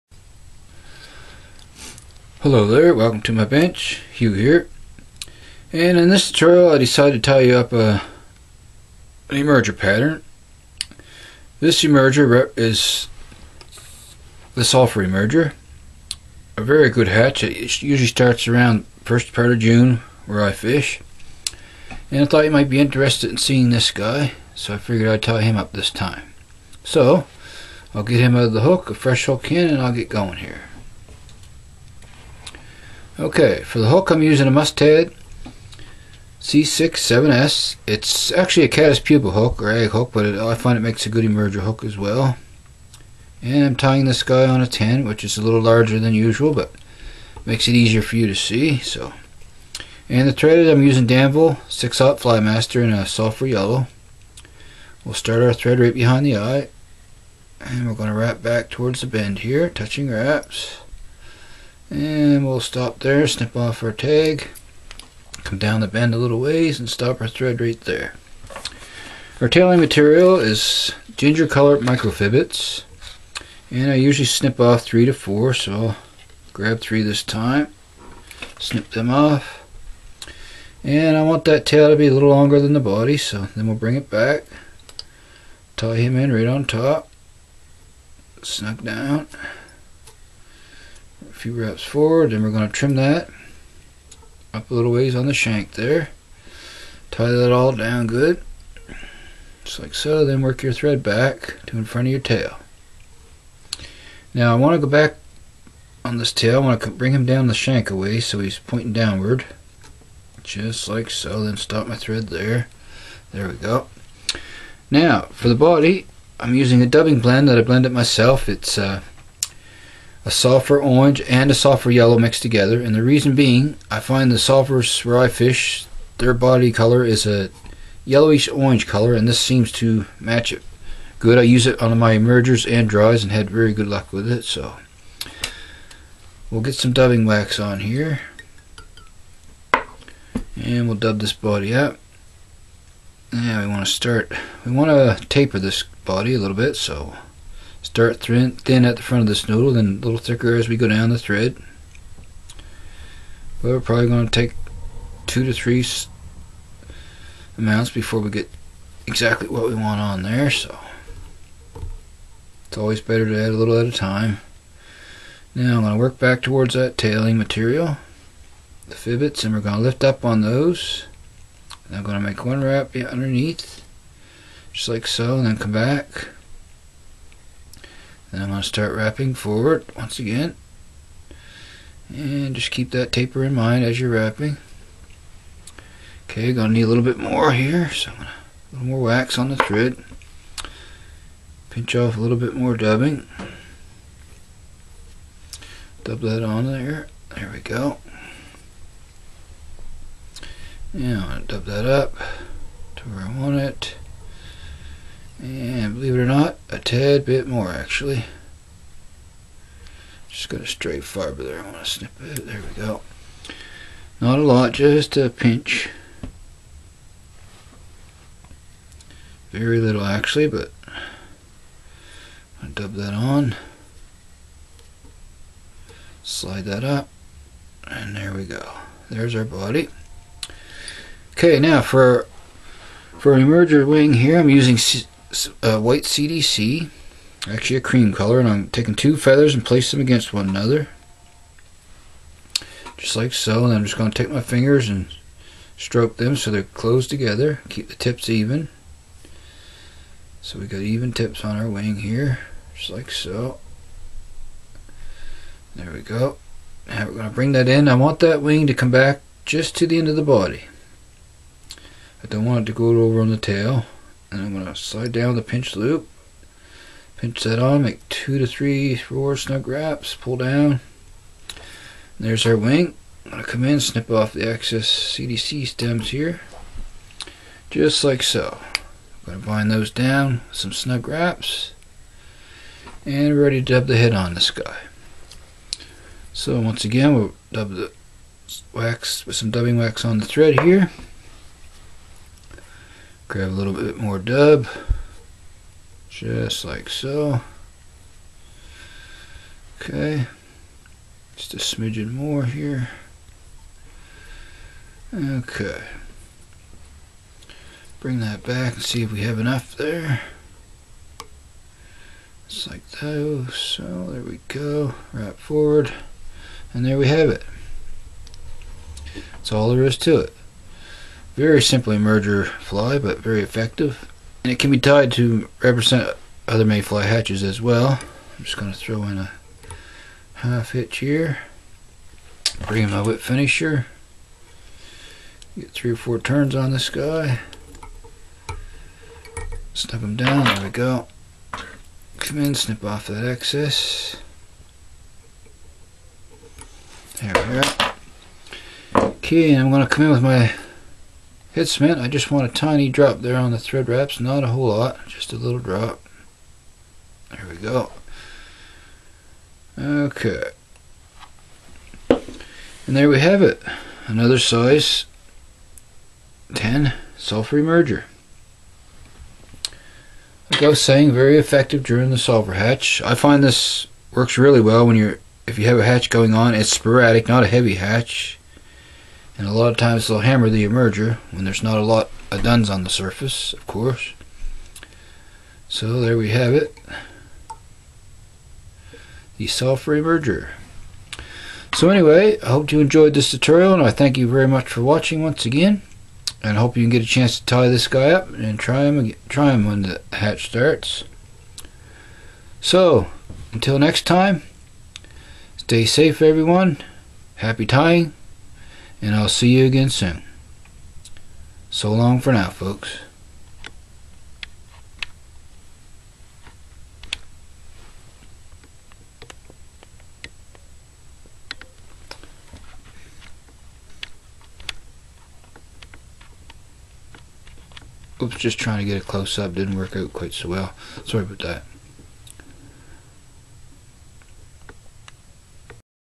fly tying with hugh! sulphur emerger .
hook mustad c67s size 12 to 18 , thread denville 6/0 sulphur yellow, tail 3 ginger micro fibettes,  body blend of yellow orange sulphur dubbing, wing white cdc  head same as body.